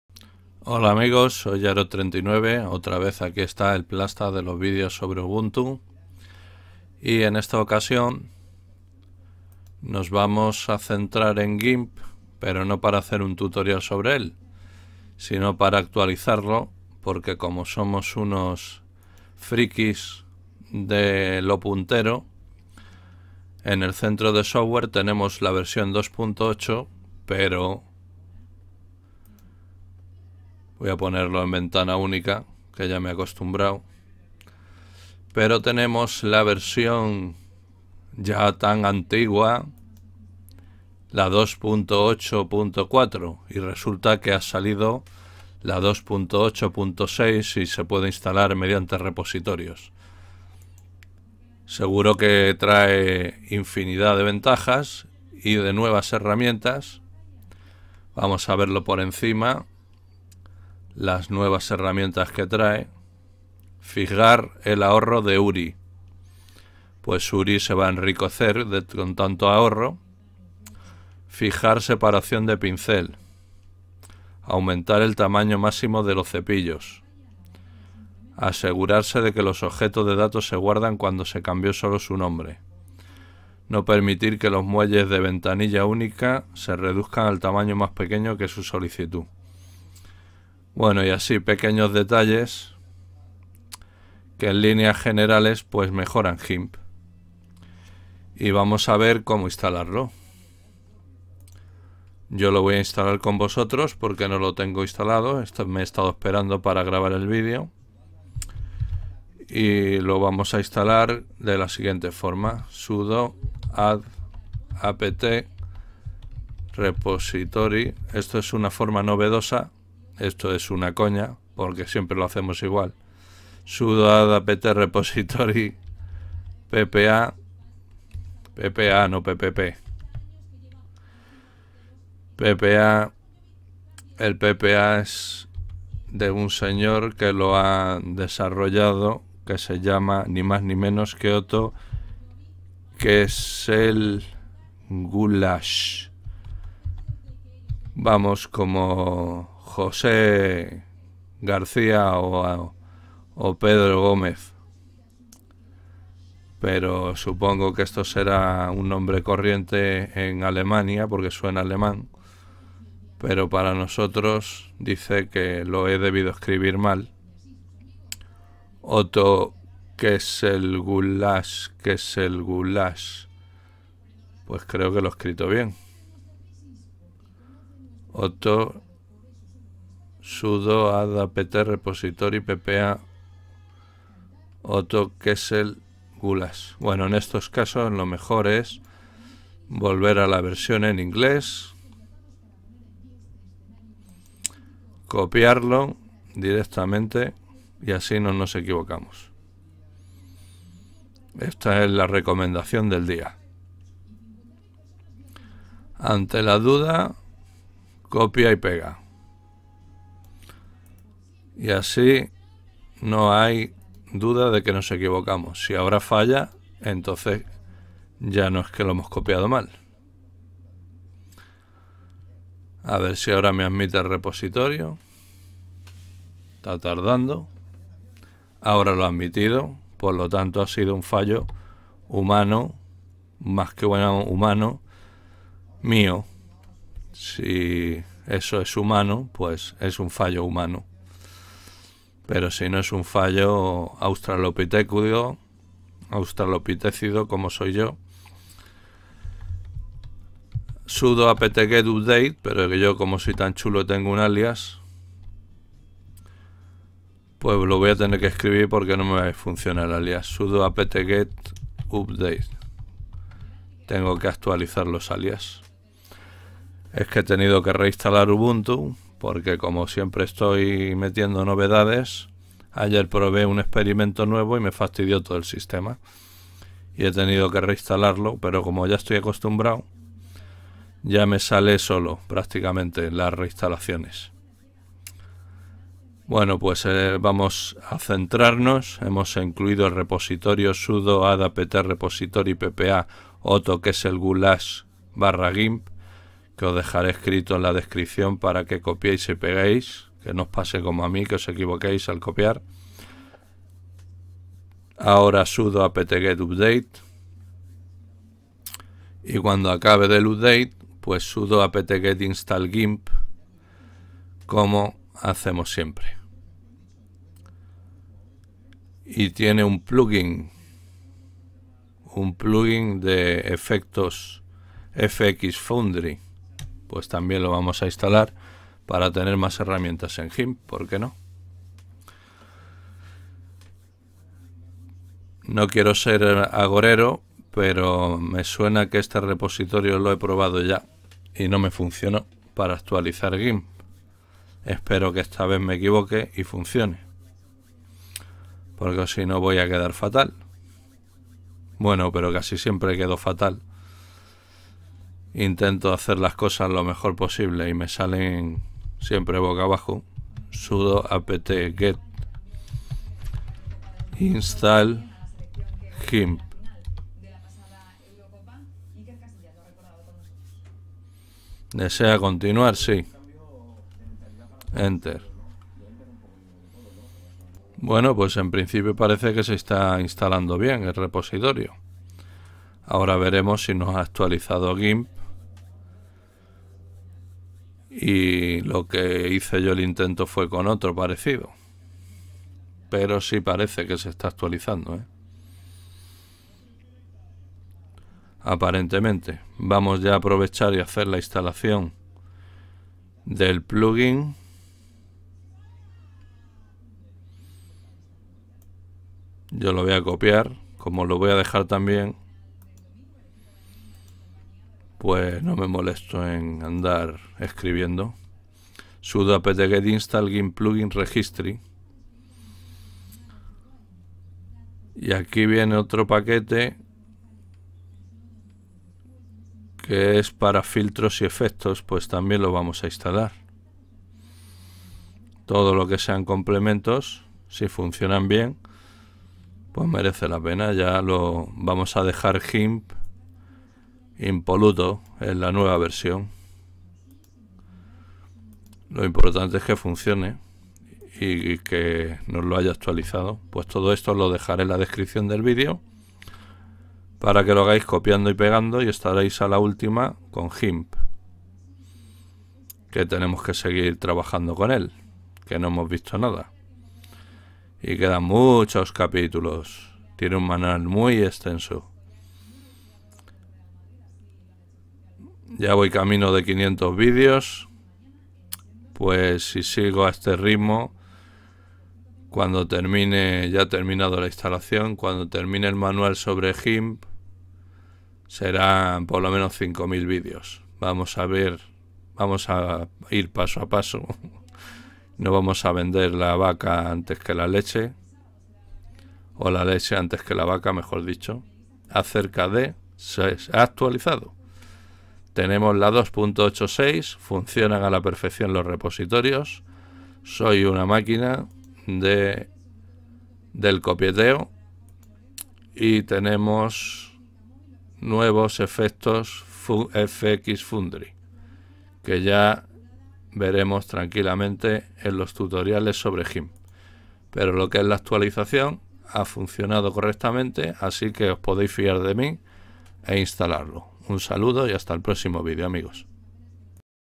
TODO UBUNTU : ACTUALIZACIÓN DE GIMP 2.8.6
sudo add-apt-repository ppa:otto-kesselgulasch/gimp
sudo apt-get update
sudo apt-get install gimp
sudo apt-get install gimp-plugin-registry
sudo apt-get install gimp-gmic